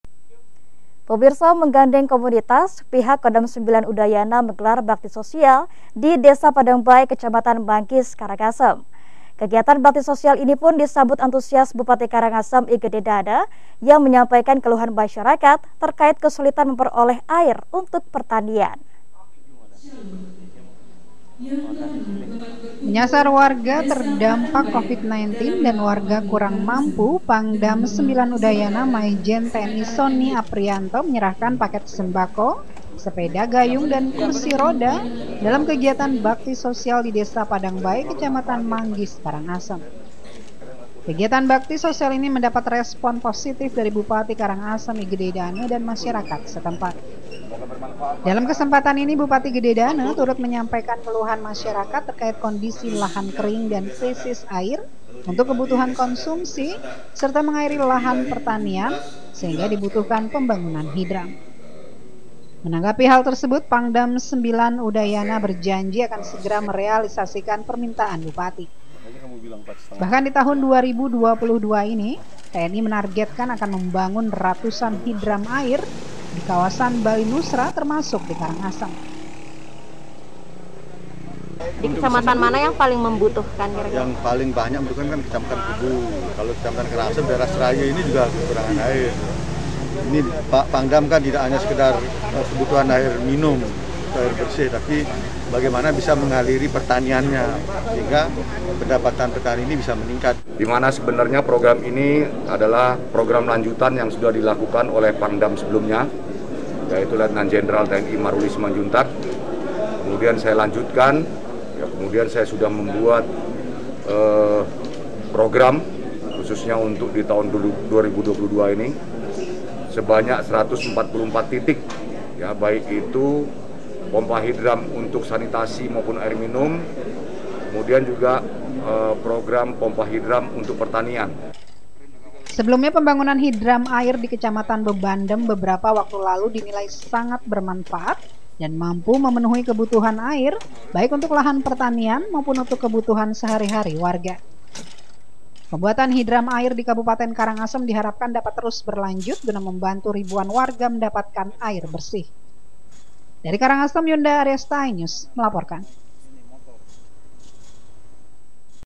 Atasi Kesulitan Air Bersih, Pangdam IX Udayana Janjikan Pembuatan Hidram di Manggis Karangasem
iNews Bali 17 Maret 2022, Atasi Kesulitan Air Bersih, Pangdam IX Udayana Janjikan Pembuatan Hidram di Manggis Karangasem